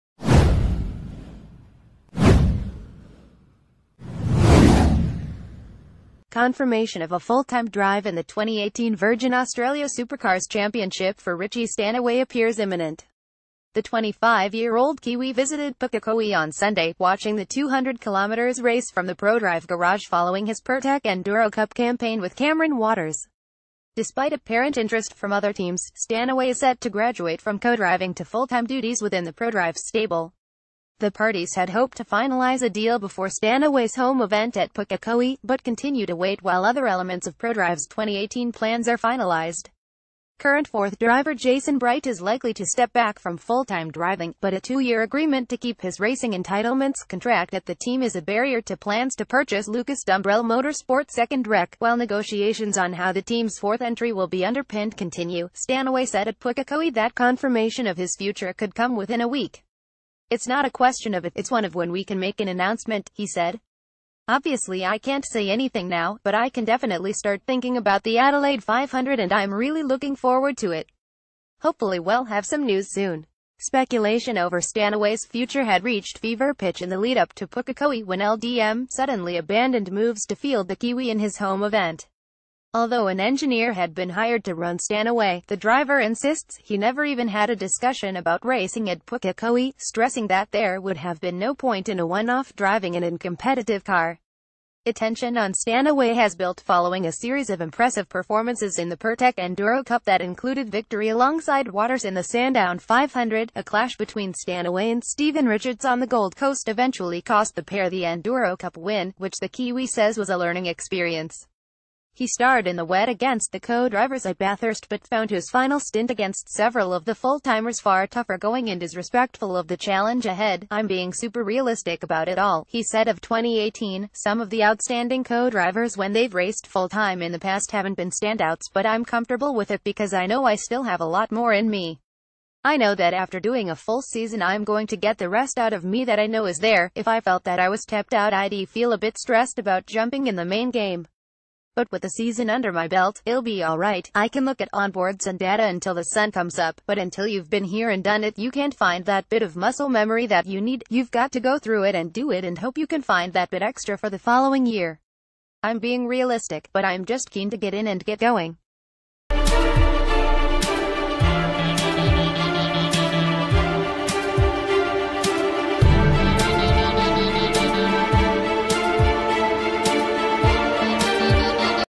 Breaking News | 'when, not if' for full-time 2018 stanaway deal
'when, not if' for full-time 2018 stanaway deal

Confirmation of a fulltime drive in the 2018 Virgin Australia Supercars Championship for Richie Stan...